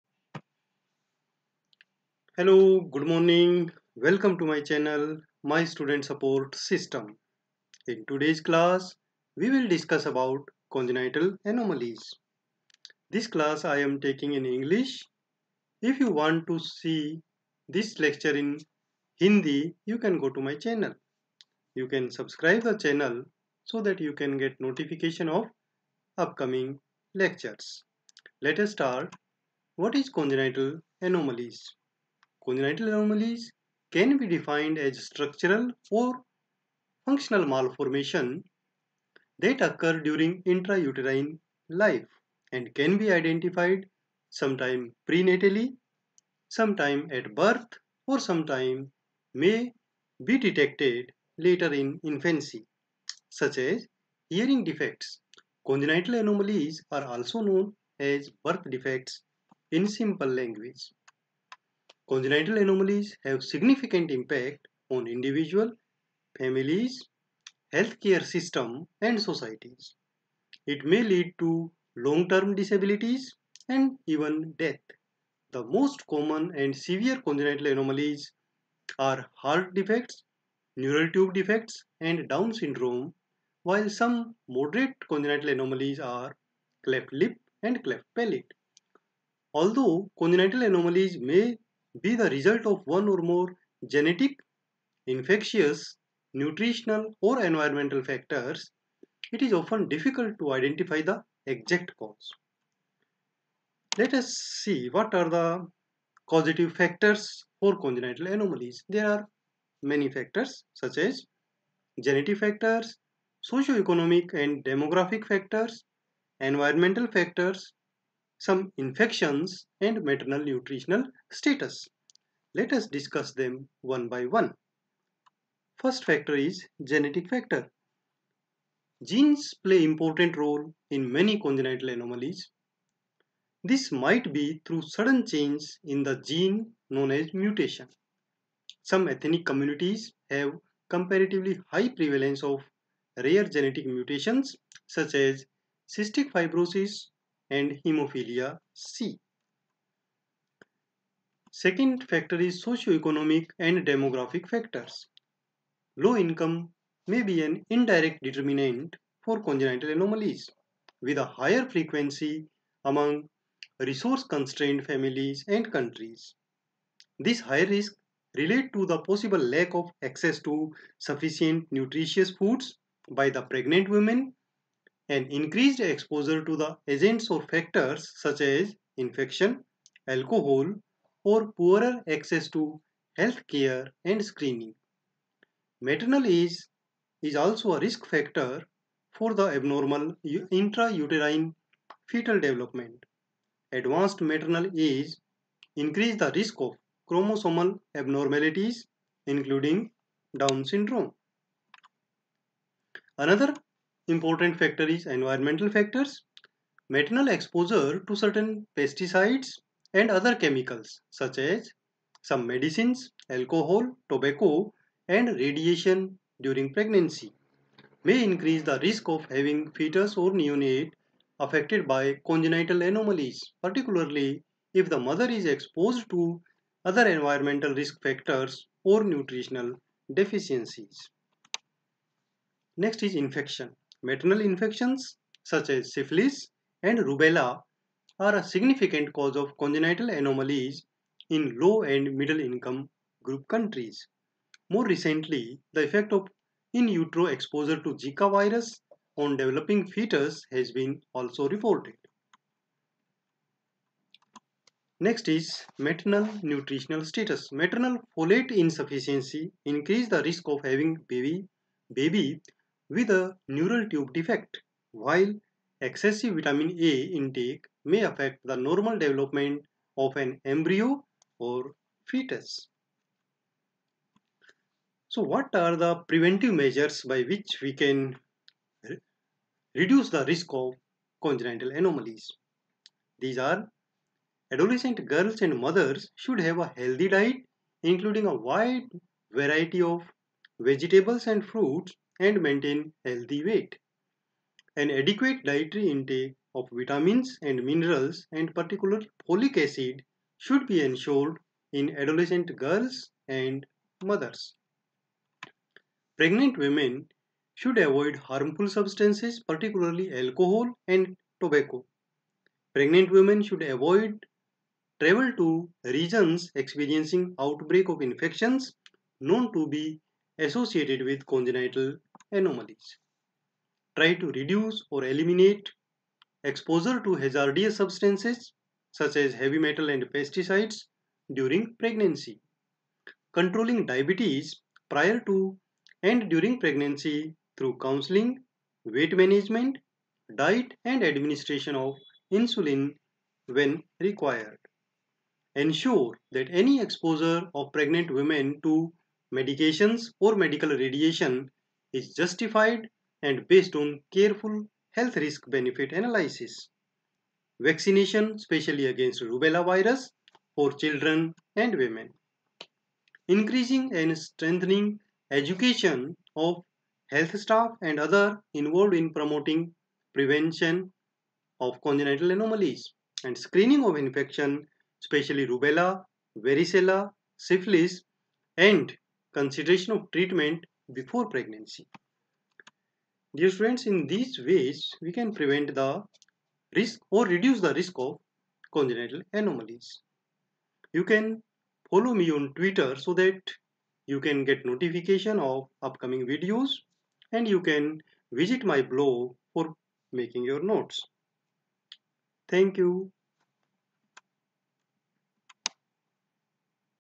CONGENITAL ANOMALIES ENGLISH | janamjaat vikritiyan
THIS VIDEO EXPLAINS ABOUT congenital anomalies in children,IN EASY WAY YOU CAN JOIN Important links- NOTES